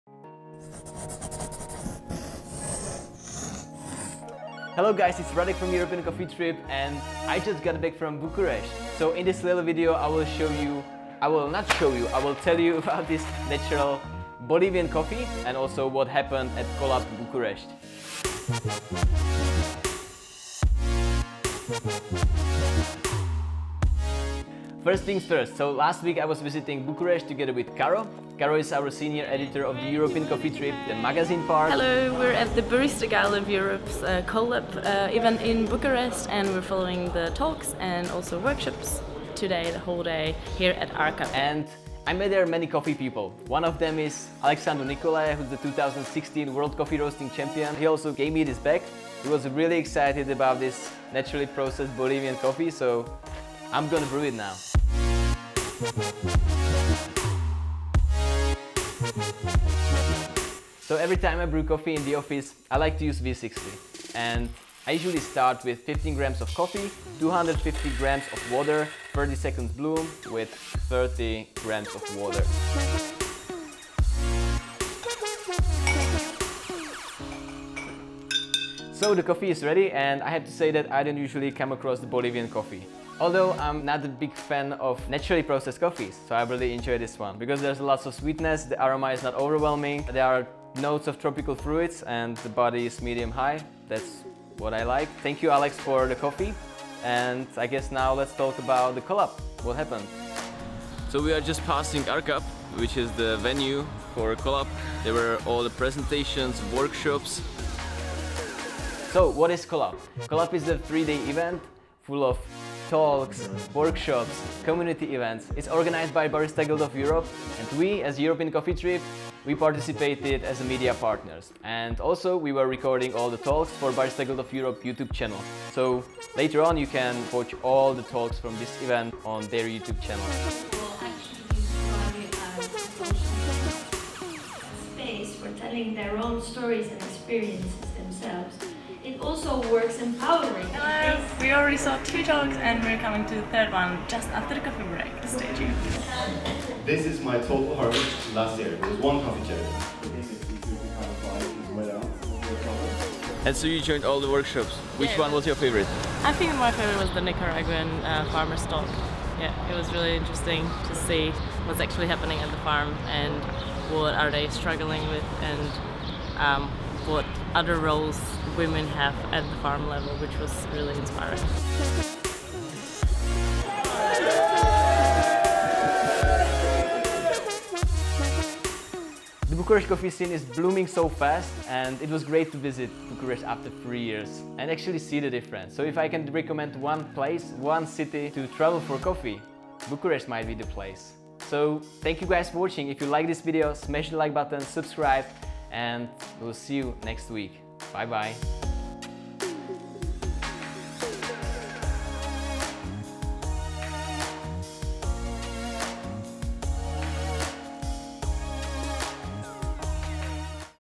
BOB Coffee Lab & CoLab: Bucharest | ECT Weekly #051
The Bucharest speciality coffee scene has an incredible vibe, so we were excited to get back after two years to meet our old friends and discover some new places. The main reason for our visit was CoLab: Bucharest organised by Barista Guild of Europe.

Back in the office, Radek tasted delicious naturally processed Bolivian coffee roasted by the 2016 World Coffee Roasting Champ, Alexandru Nicolae, at BOB Coffee Lab.

European Coffee Trip is an online magazine dedicated to speciality coffee culture in Europe.